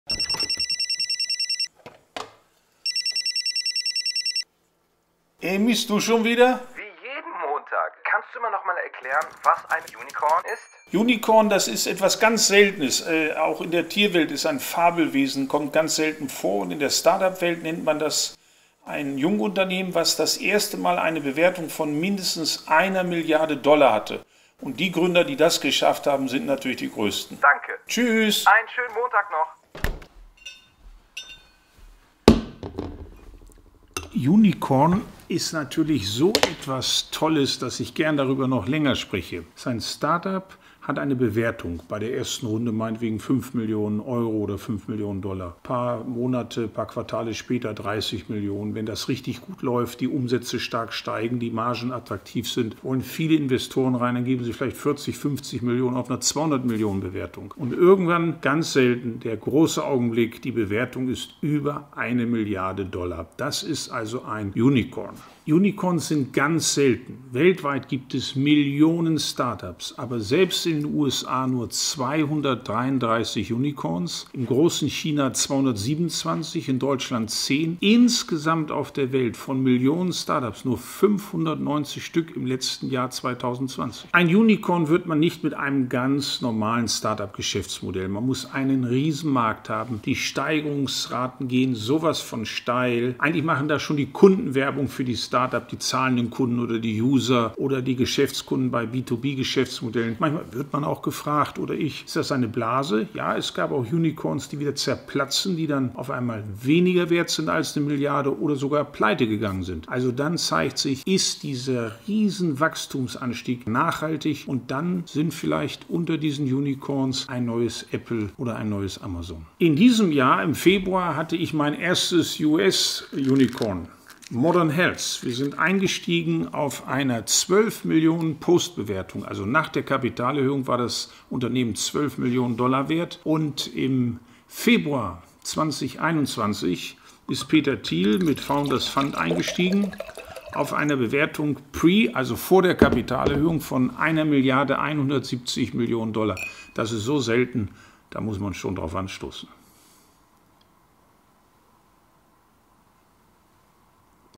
Was ist ein Unicorn? - Carsten Maschmeyer erklärt Gründungsbegriffe
Wieder Montag und wieder ein neuer Begriff. Heute: Unicorn. Was es mit diesen Fabelwesen in der Gründungswelt auf sich hat, erkläre ich nicht nur Euch, sondern natürlich auch meinem Lieblingsmoderator @amiaztube. Und ich stelle Euch Modern Health ein bisschen vor - unser erstes Unicorn der Maschmeyer Group.

• Was für Videos würdet Ihr gerne in Zukunft von mir sehen?
• Wie findet Ihr die aktuelle Staffel von DHDL?

Schreibt es mir gerne in die Kommentare! 👇